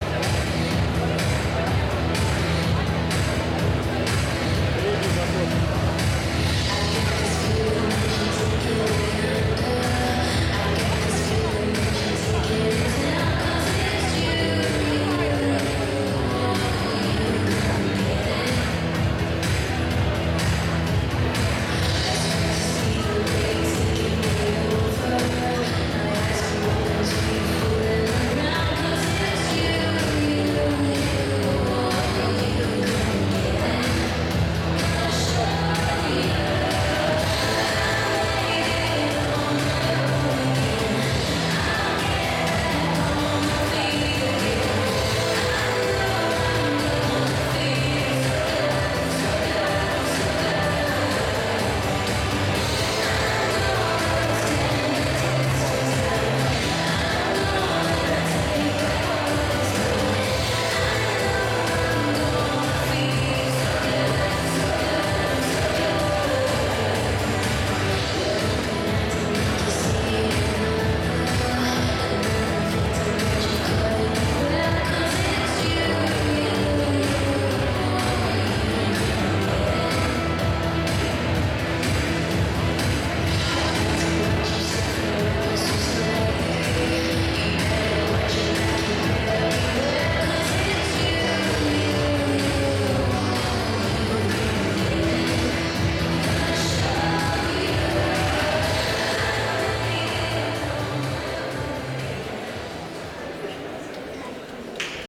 17.9.16 Открытие сезона 1/4 E-class заход 3
Открытие сезона по хастлу и дискофоксу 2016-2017 года
17 сентября2016 г.
Место проведения: ТДЦ Буревестник
Организаторы проведения: Ассоциация Спортивного Хастла
Непосредственные организаторы: Хастл-центр, Елена Кетуровская-Козлова, Андрей Григорьев и Ко
видеооператор: Игорь Скобелев